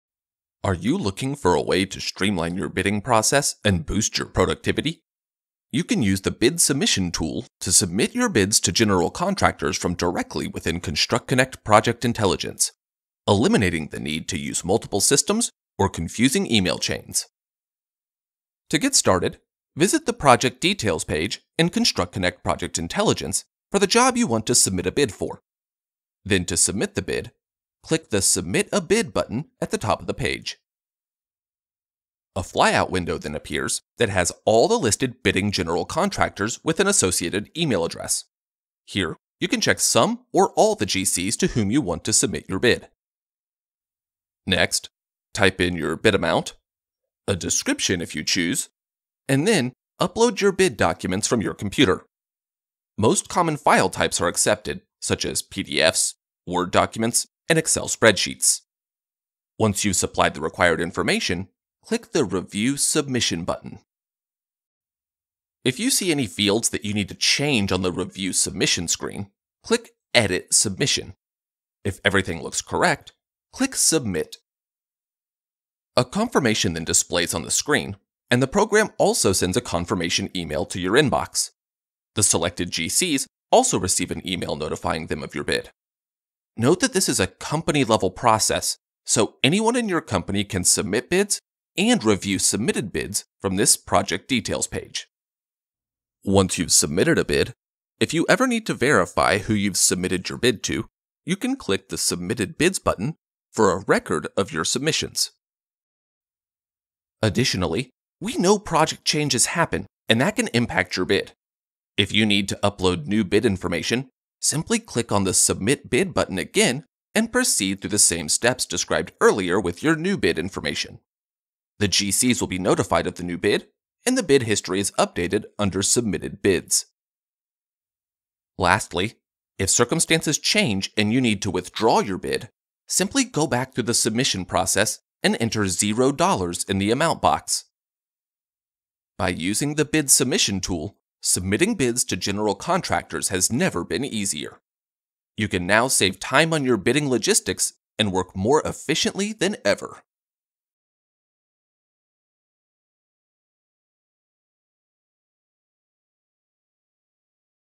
Bid Submission for Jobs Found in ConstructConnect Project Intelligence
In this video, discover how to streamline your bidding process with the bid submission tool in Construct Connect Project Intelligence. Learn how to submit bids directly to General Contractors, manage bid details, and efficiently update bid information as projects evolve. Save time and enhance productivity with step-by-step guidance on submitting, reviewing, and withdrawing bids seamlessly.